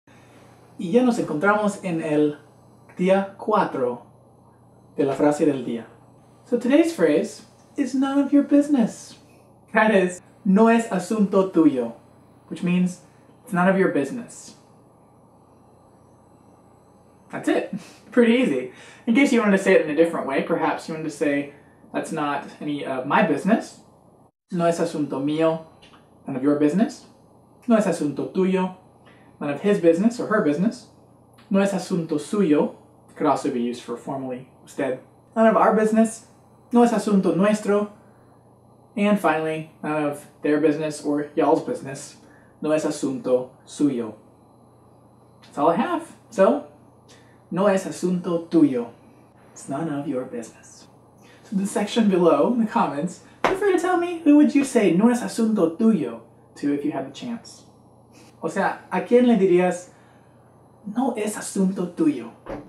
How to say, "It's none of your business!" in Spanish (Día 4)
Practice Your Spanish With Chateo - a Spanish-speaking chatbot!  | Get a discount off your first 6 months with code CHATEOBOT | Sign up

Wow.  Ya estamos en el cuarto día del proyecto.  Ojalá les guste.